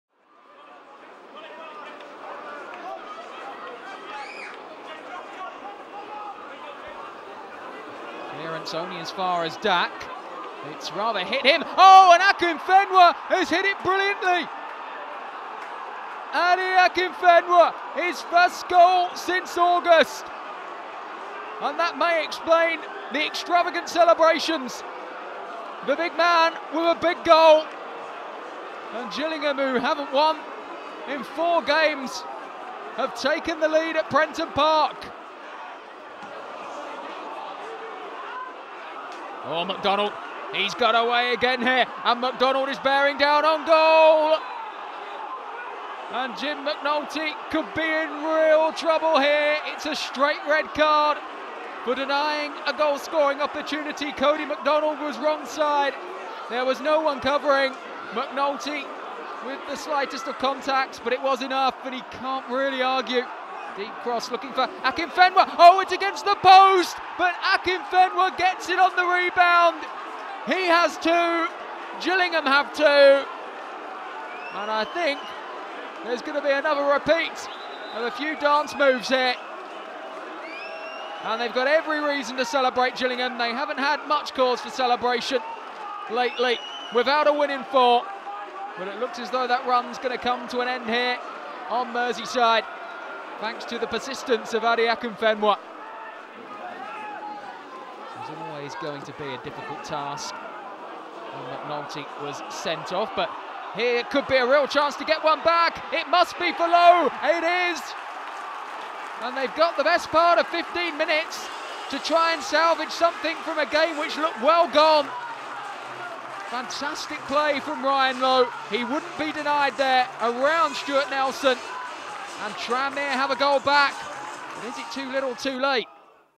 Tranmere Rovers vs Gillingham - League One 2013/14 Highlights
Tranmere Rovers 1-2 Gillingham.

Highlights from Tranmere's Sky Bet League One match against Gillingham.